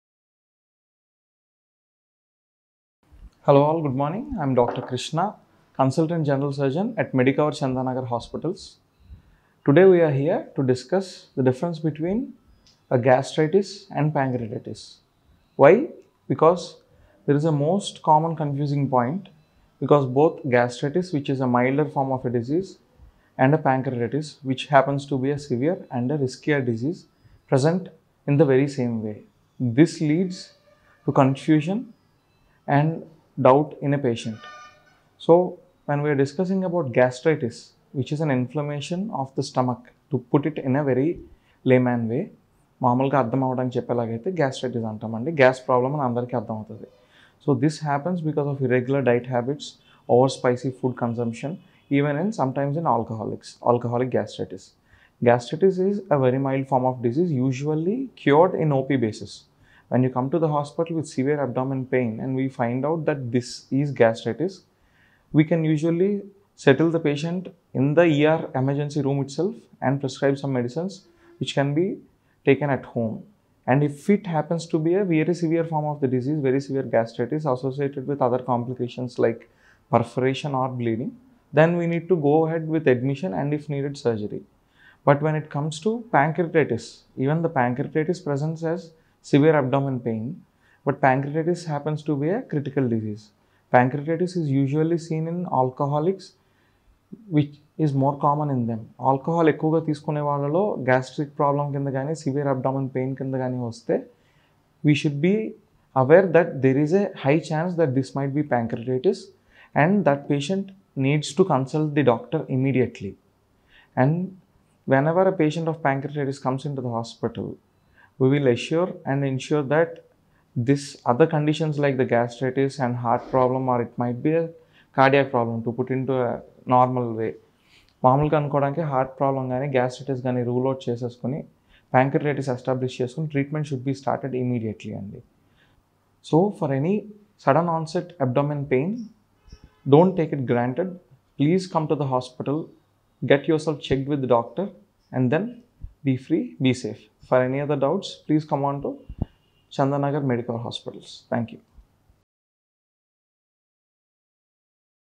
Difference Between Gastritis And Pancreatitis | Gastritis Symptoms
Dr. M. Krishna Bhargav, Consultant General Surgeon, Medicover Hospitals Chandanagar, Explained briefly the difference between Gastritis and Pancreatitis.

0:00 - The difference between Gastritis and Pancreatitis.

This is the most common confusing point because gastritis is the milder form of disease and pancreatitis is a severe disease, and they both are present in the very same way. This leads to confusion and doubt in the patients.

0:34 - Gastritis:
Gastritis is an inflammation of the stomach. It is a very mild form of the disease usually cured on a Day Care basis.

0:47 - What causes Gastritis?
-Irregular Diet Habits
-Over Spicy Food Consumption
-Sometimes in Alcoholics

1:25 - Pancreatitis:
Pancreatitis is inflammation in the pancreas, Even pancreatitis presents as severe abdominal pain but pancreatitis happens to be a critical disease. Pancreatitis is usually seen in alcoholics.

To know more about Gastritis and Pancreatitis, Check out this video.